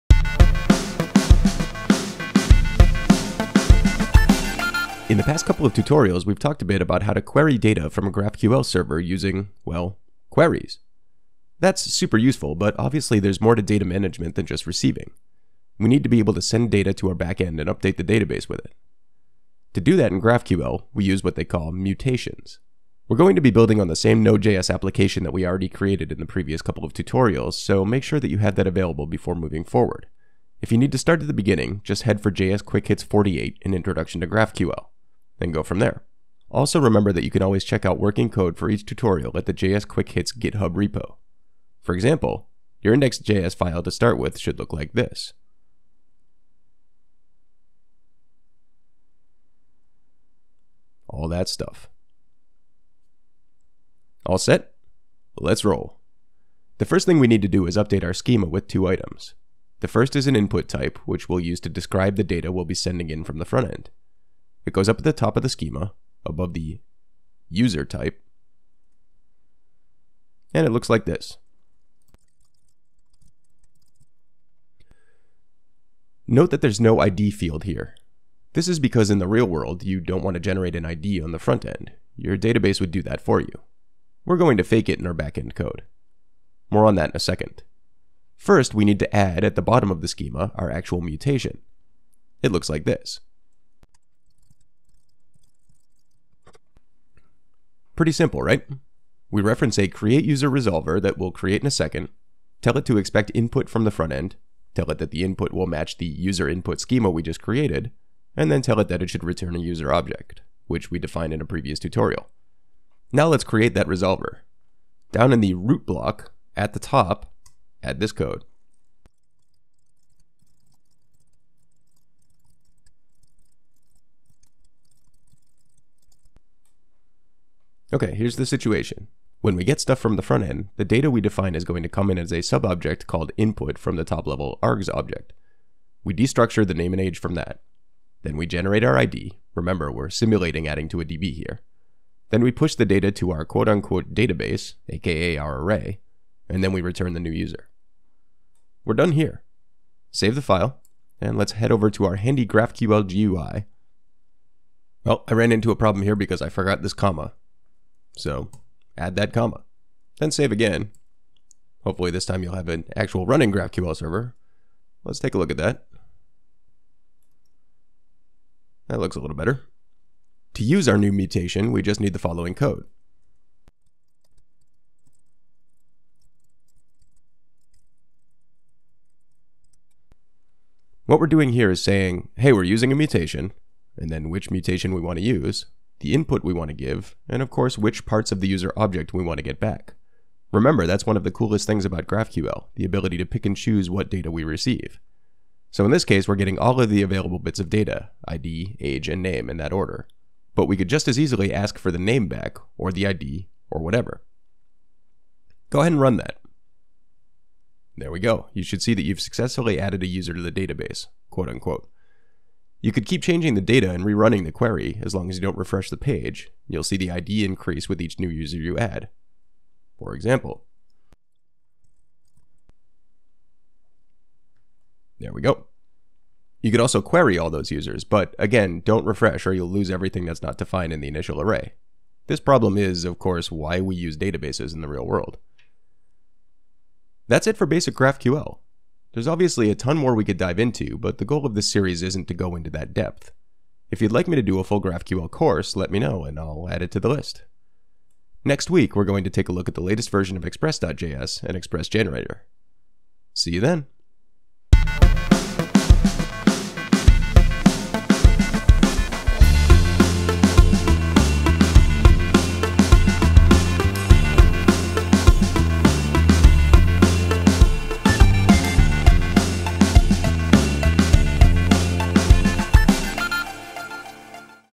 JS Quick Hits 51 - GraphQL Mutations
Now that we know how to query our DB with GraphQL, let's take a look at how to send changes with mutations!